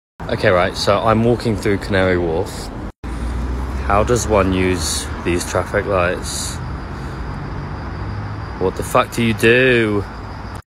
Canary Wharf Traffic Light Display Confuses Onlooker || ViralHog
Occurred in August 2021 / London, England, UK

"The video was taken at Canary Wharf in London at a traffic light art display"

Credit: TikTok/IG @elliott.unger